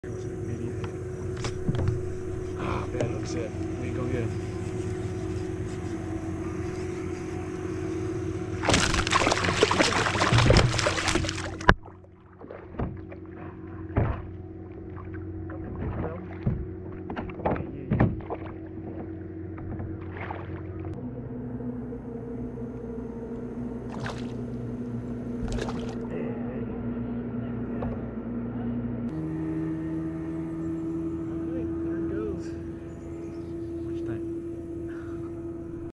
Almost lost my paddle! ( kayak bass fishing w/ Nikko Hellgrammite )
Almost lost my paddle trying to get this bass in the boat. Was throwing a Nikko Hellgrammite on the Tokyo rig.

kayak bass fishing
kayak fishing
structure fishing
Tokyo Rig